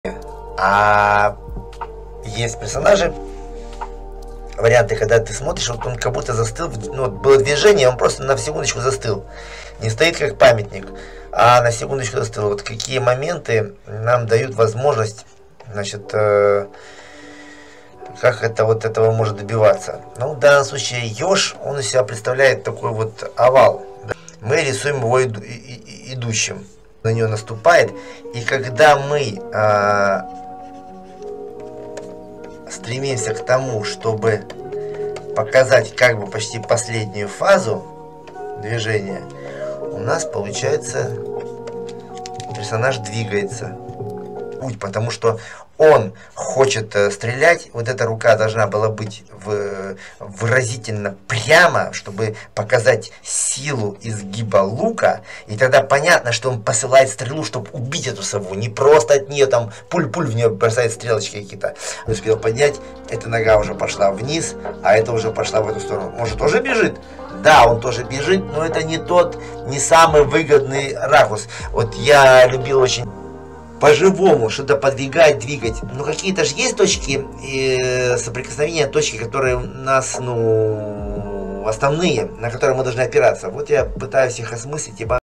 как придать движение персонажу в иллюстрации
Первый видеоролик в серии "Как сделать персонажа живым,как придать динамику и движение"  Записал видеоурок,выложил на boosty .Рассказываю ,какими приемами и логикой пользуюсь при передаче движения персонажа..Это не одно видео . Всего лишь первое.Полное видео на